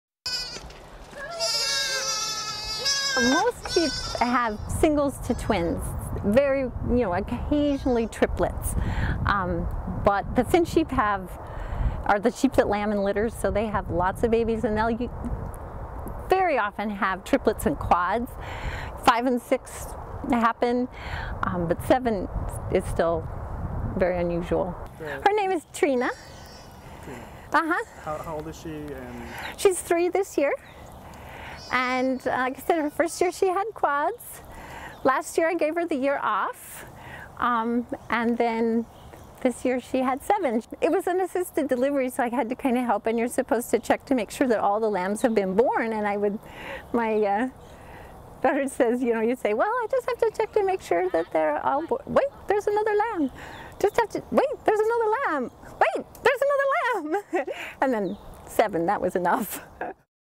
A 3-year-old ewe in Washington state has done something that rarely happens; given birth to a litter
(31 May 2012) HEADLINE:  Unusual litter: Ewe gives birth to 7 lambs in Wash.
CAPTION: A 3-year-old ewe in Washington state has done something that rarely happens; given birth to a litter of 7 lambs.  The American Finnsheep Breeder's Association says that's only been recorded twice before in the U.S.  (April 13)
[Location - Date:SNOQUALMIE, WASHINGTON -- AP VIDEO --APRIL 13]
[Notes:SOT -- COLLEEN PECK / EWE'S OWNER]
most sheep have singles to twins...occasionally triplets. finnsheep have lots of babies..often have.triplets and quads. five and six happen. but seven is still unusual. her name is trina. she's three...first year have quads.
Last year i gave her the year off. then had seven...it was assisted delivery...you have to check to see all lambs have been born. my daughter says...i have to check to make sure they are all here...wait there is another lamb...wait there is another lamb...seven. that's enough."